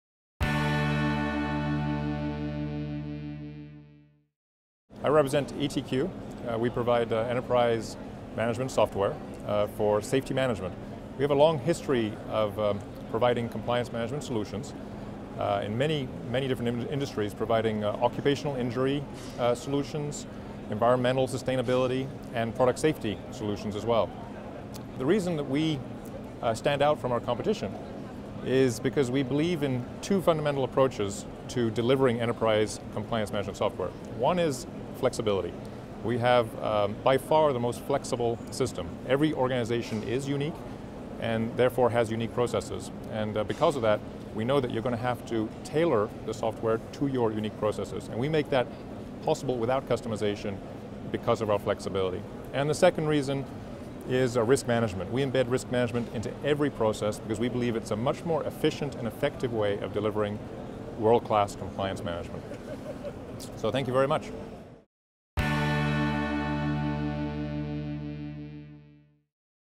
EtQ enterprise management software for safety management
EtQ on how flexibility & risk assessment enhance safetymanagement software.@etqinc